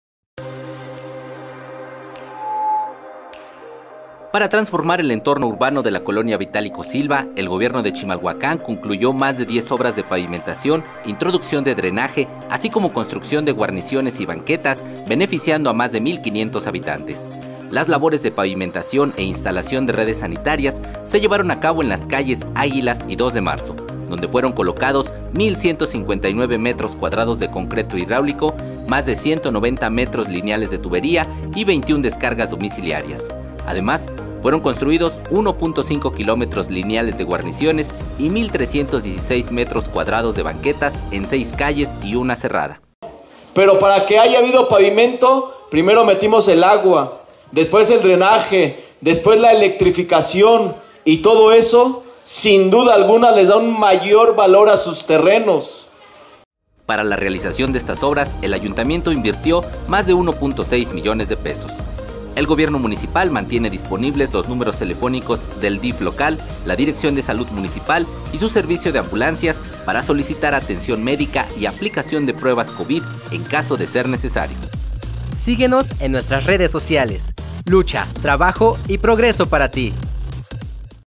Mejoramos entorno urbano de la colonia Vitálico Silva
Para transformar el entorno urbano de la colonia Vitálico Silva, el gobierno de Chimalhuacán, concluyó más de diez obras de pavimentación, introducción de drenaje, así como construcción de guarniciones y banquetas, beneficiando a más de 1,500 habitantes.

Las labores de pavimentación e instalación de redes sanitarias se llevaron a cabo en las calles Águilas y 2 de Marzo, donde fueron colocados 1,159 metros cuadrados de concreto hidráulico, más de 190 metros lineales de tubería y 21 descargas domiciliarias

Además, fueron construidos 1.5 kilómetros lineales de guarniciones y 1,316 metros cuadrados de banquetas en seis calles y una cerrada.